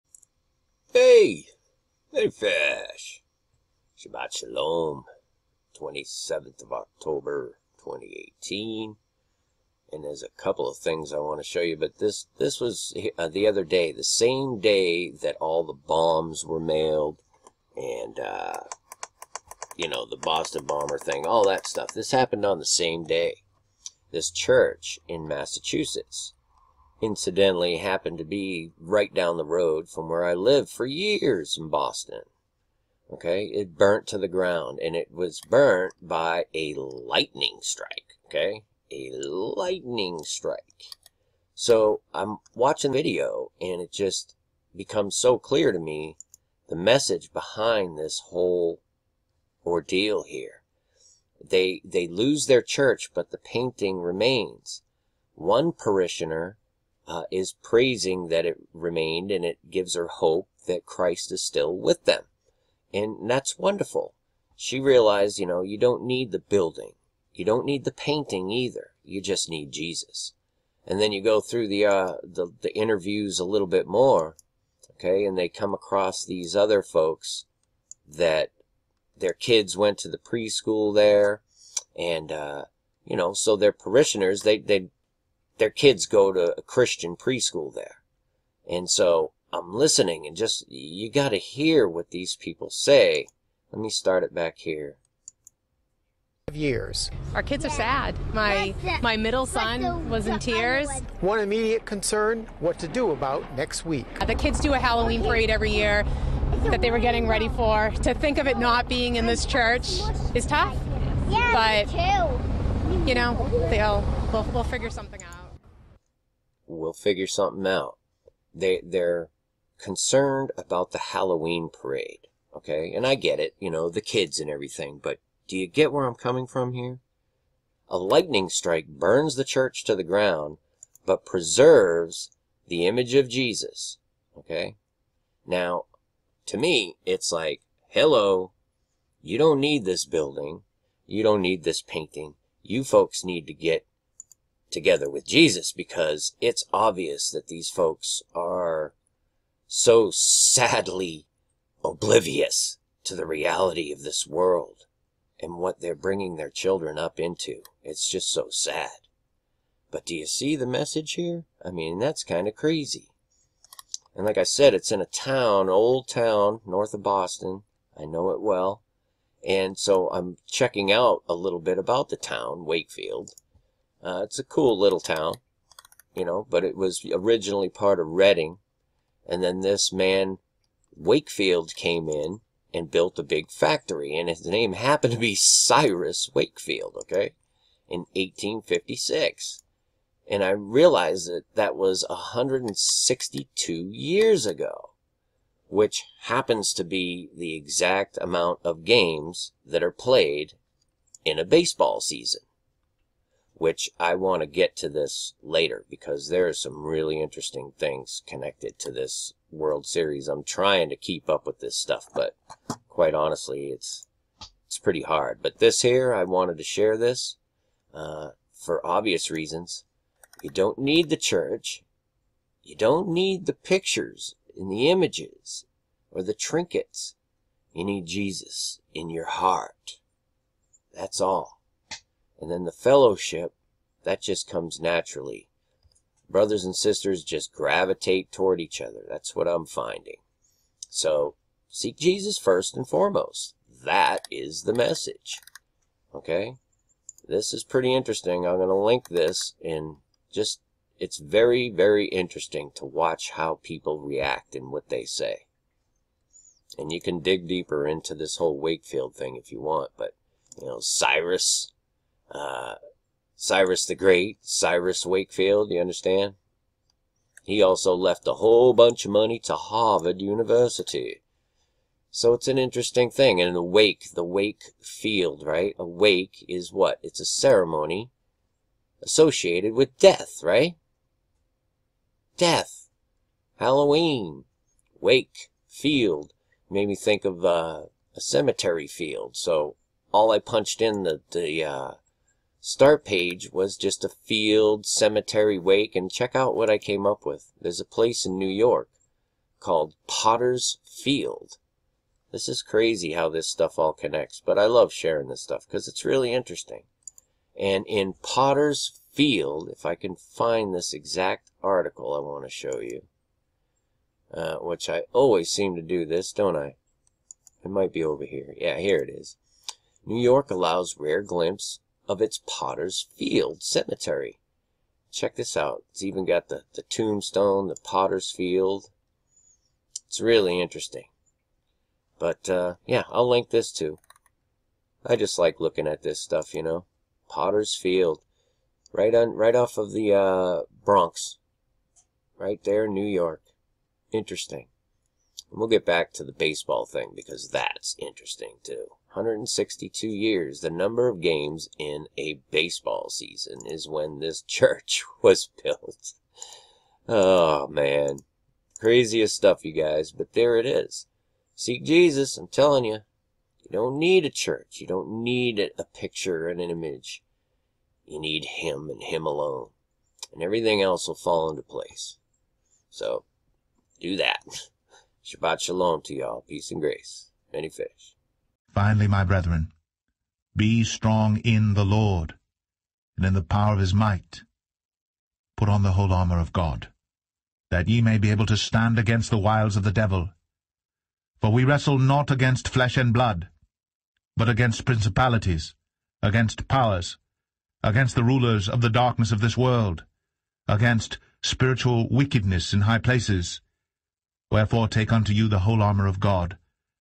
Church Destroyed by 'Explosive 'Lightning 'Fire! Jesus Survives! Will You Survive the Coming Fire?
Jesus went through the fire for us! Remain in Him and He will carry you through the fiery furnace! Praise God! Thank you Jesus! Shalom to you all!!

Amen.